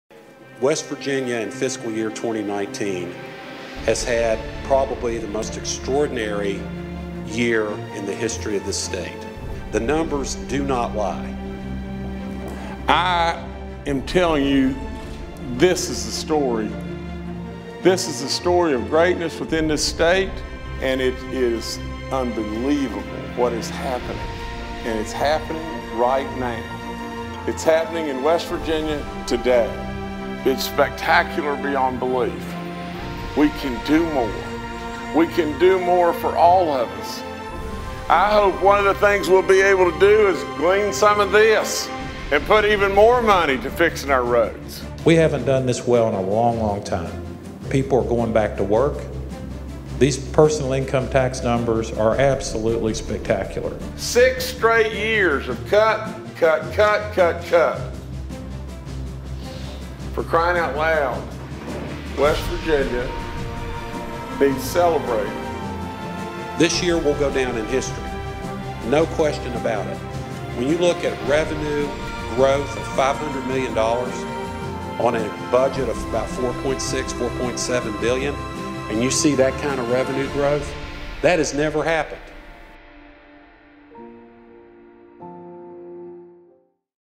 Recap: Gov. Justice announces all-time state record for revenue growth set in FY2019
With the twelfth and final month of Fiscal Year 2019 now in the books, Gov. Jim Justice announced that general revenue growth of $511 million for the year marked the greatest single-year revenue growth total in West Virginia history.

For perspective, the $511 million in revenue growth for FY2019 alone is more than one-and-a-half times the combined revenue growth in West Virginia for the entire decade from FY2007 to FY2017.